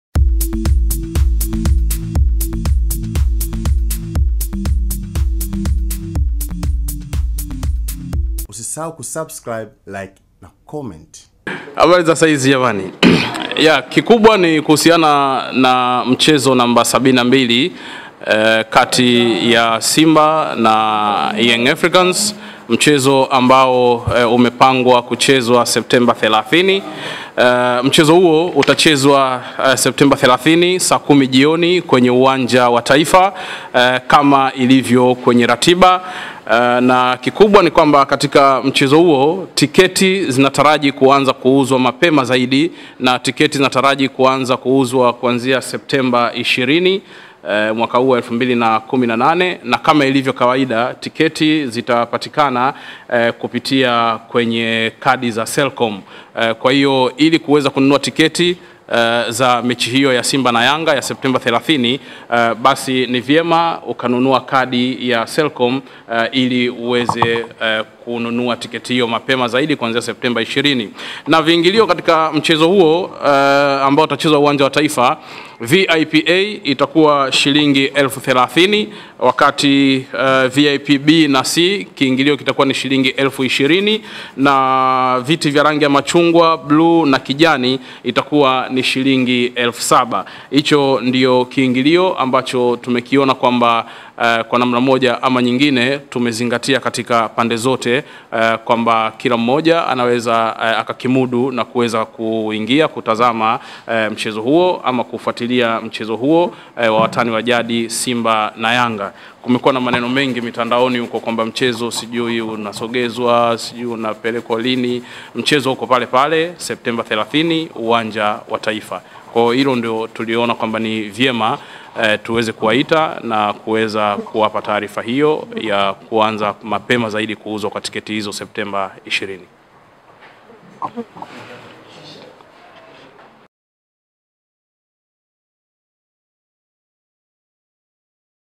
TFF KUTOA ANGALIZO KUELEKEA MECHI WA SIMBA NA YANGA
TFF Kwa kupitia  msemaji wake Bw. Cliford Mario Ndimbo kutoa angalizo mechi ya wapinzani wa jadi Simba SC na Yanga FC Tarehe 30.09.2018